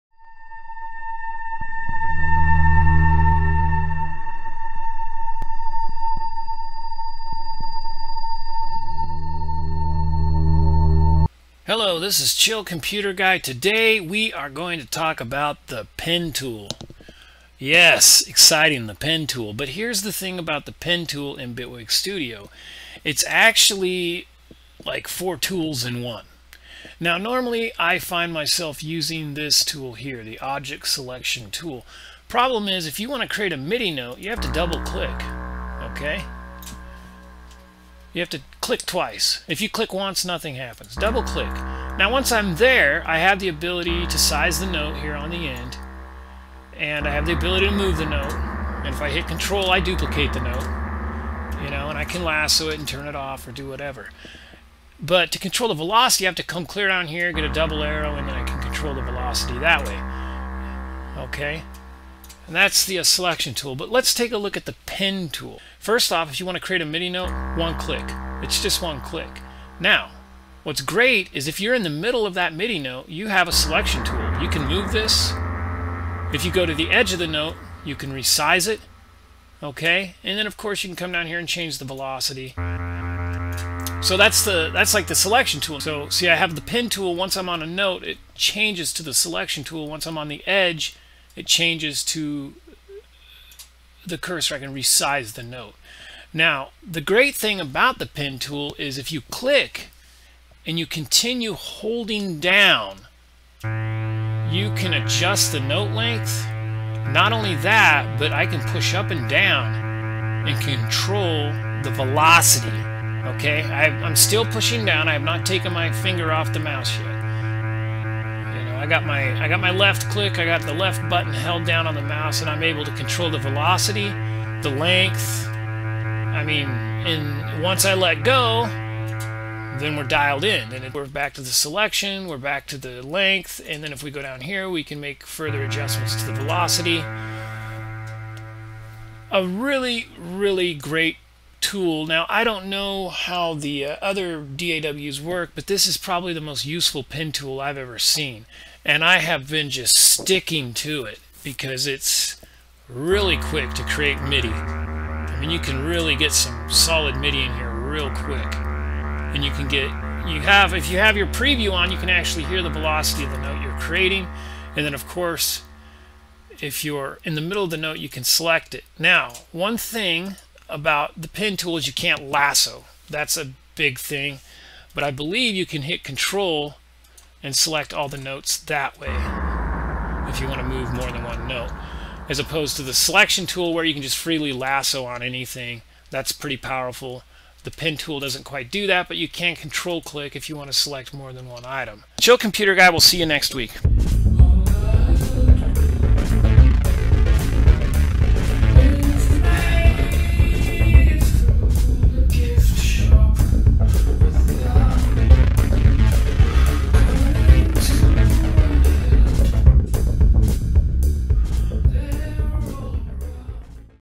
Bitwig Quick Tips: The Pen Tool
The pen tool is a great tool to quickly write MIDI. It writes selects adjust velocity and length all in one click the key is to hold the mouse button down. It's my new go to tool when I'm in the piano roll. The Pen tool Is a powerhouse of a midi writer.

Below is a special link to my other YouTube channel.